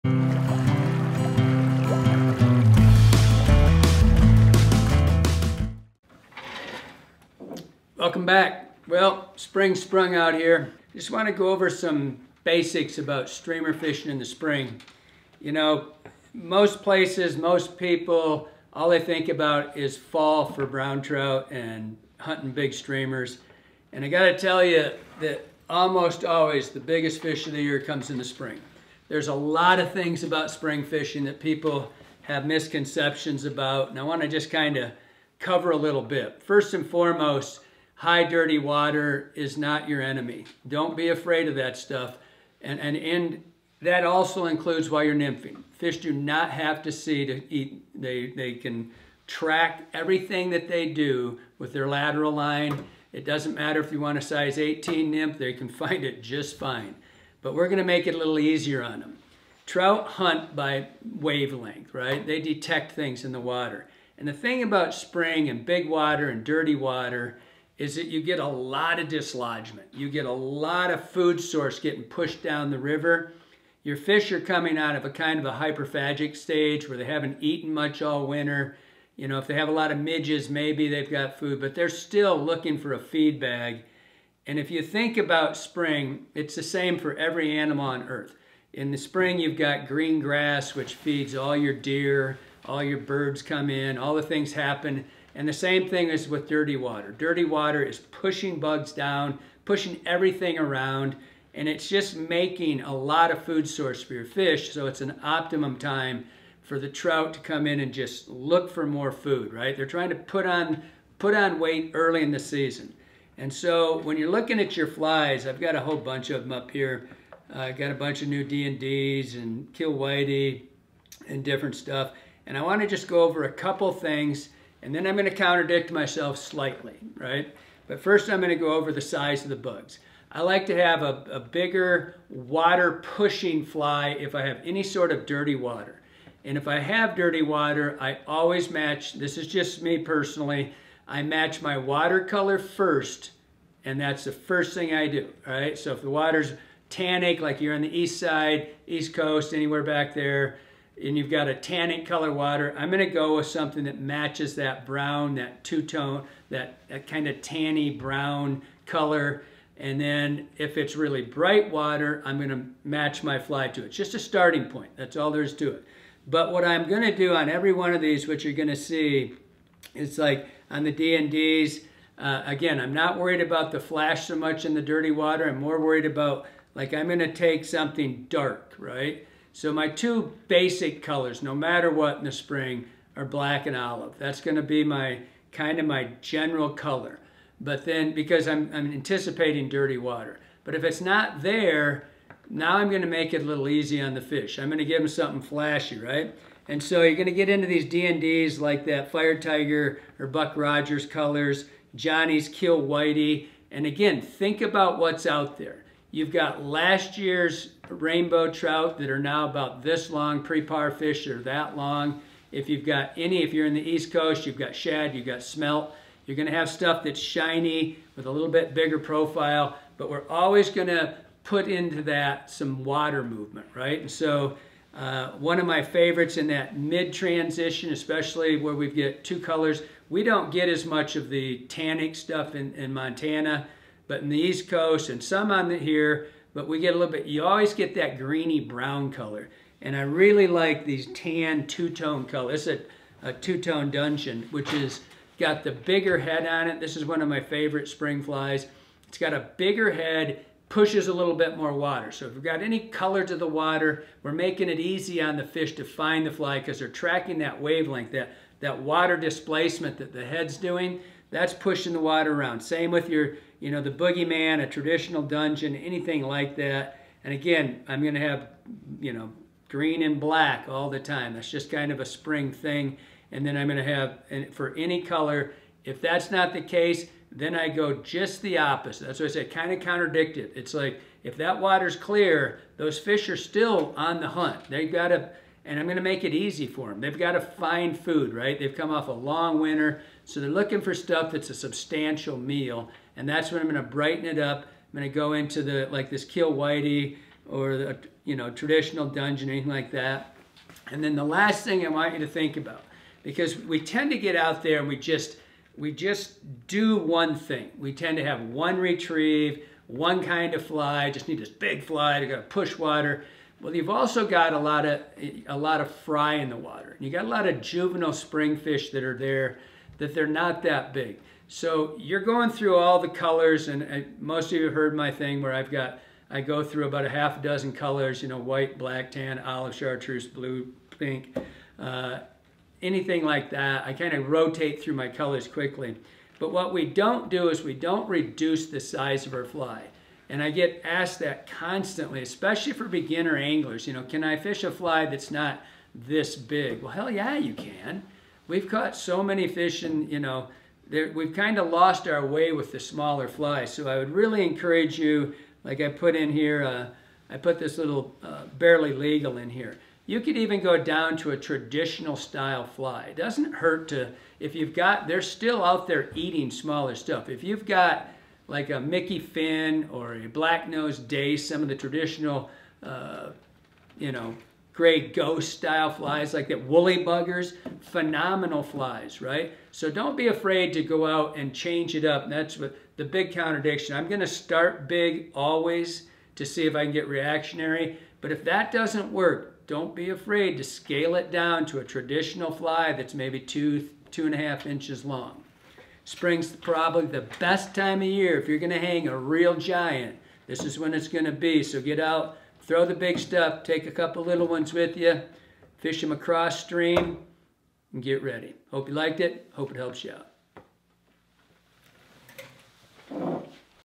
Can You Catch LARGE Brown Trout in the Spring?
Today Kelly talks us through his strategies whilst fishing in the spring. The key is to use flies with large heads that will disturb and push lots of water. Start by matching the color of the water, but if that doesn't work, try the exact opposite, then work through the rest of your colors. If you're not able to induce a reactionary take, try a smaller fly like a Black Nose Dace, a Grey Ghost, or something else in the 2-inch range.